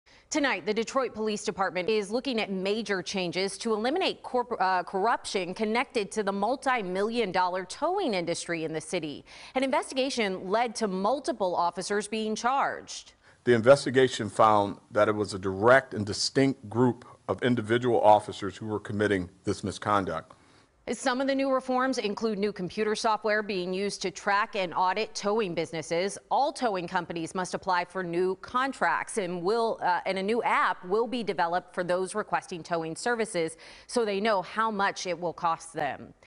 Detroit Police Department Announces Changes To Eliminate Corruption In Towing Industry
The Detroit Police Department is looking at major changes to eliminate corruption connected to the multimillion-dollar towing industry in the city.